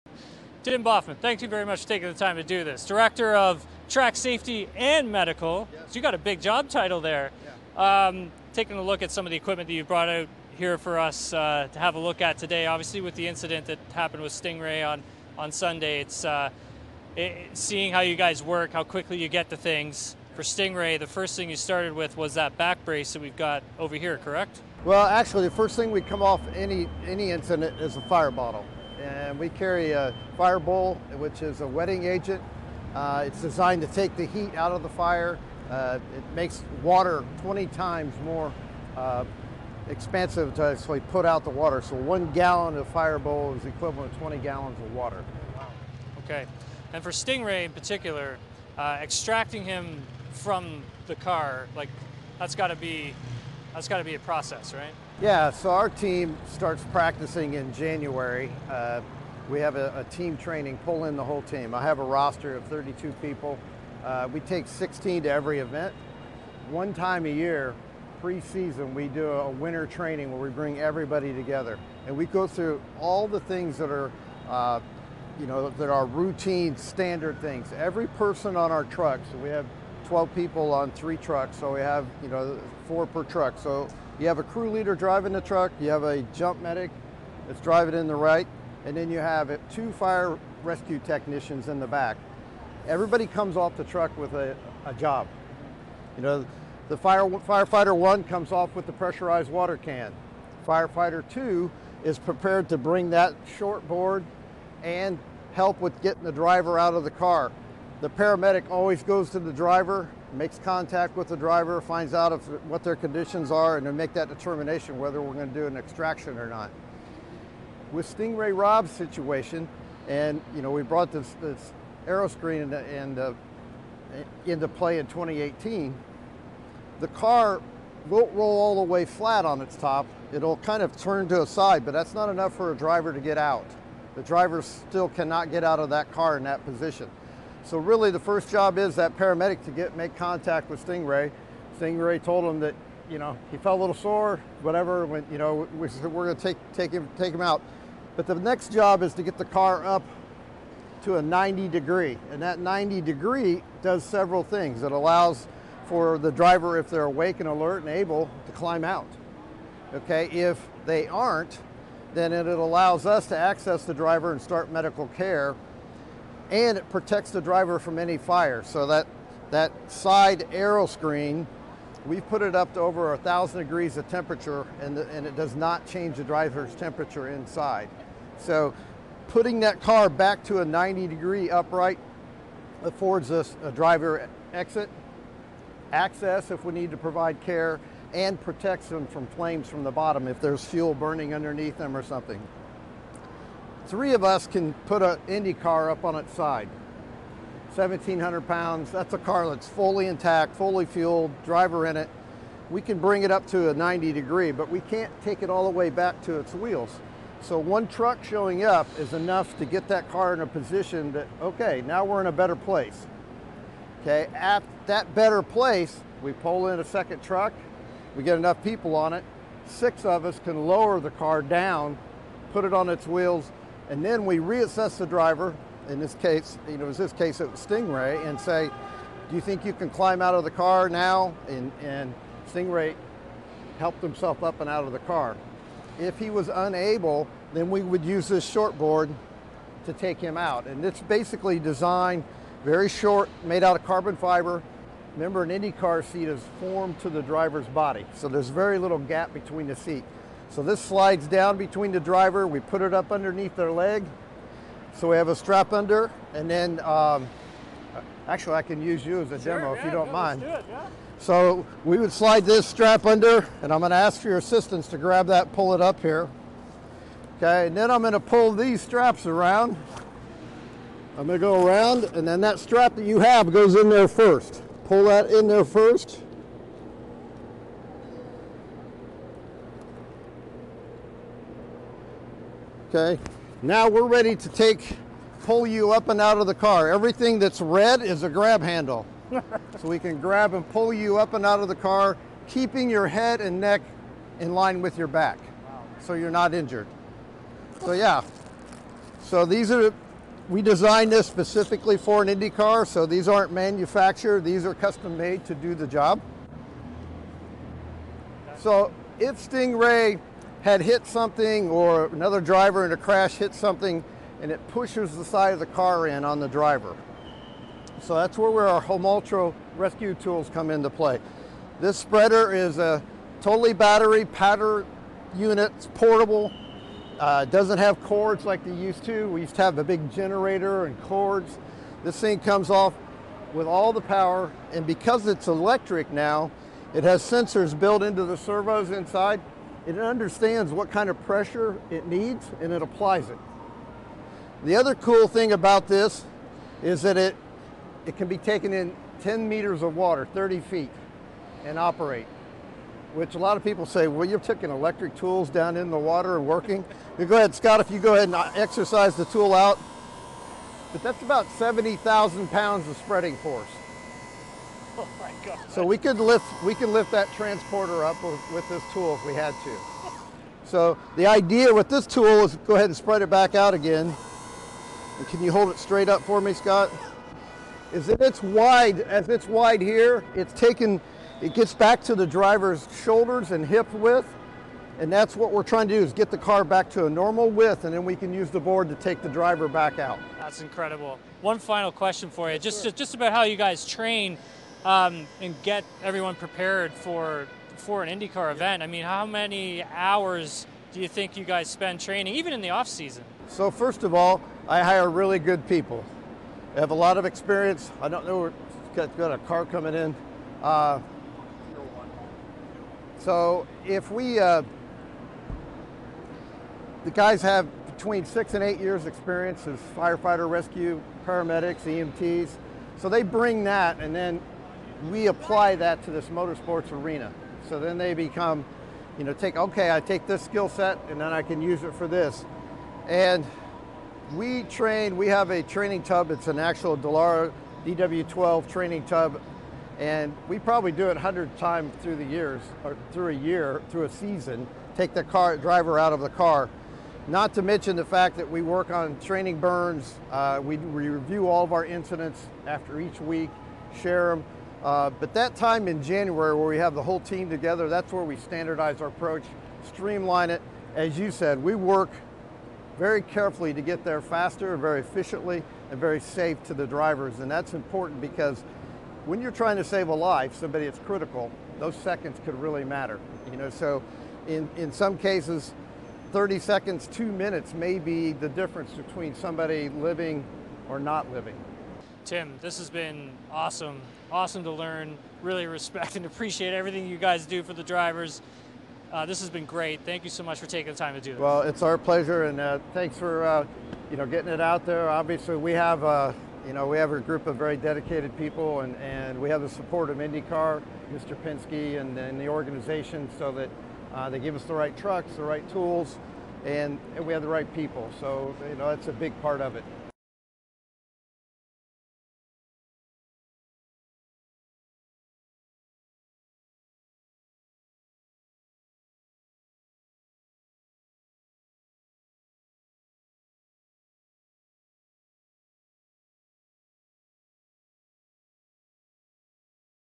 INDYCAR's track safety and medical director discusses reaction to Sting Ray Robb crash
INDYCAR senior director of track safety and medical, Tim Baughman broke down how his team reacted to the Sting Ray Robb flight and crash at Iowa Speedway. Baughman explained all the planning that goes on behind the scenes to ensure maximum safety and demonstrated an extraction technique on TSN Racing analyst Tim Hauraney.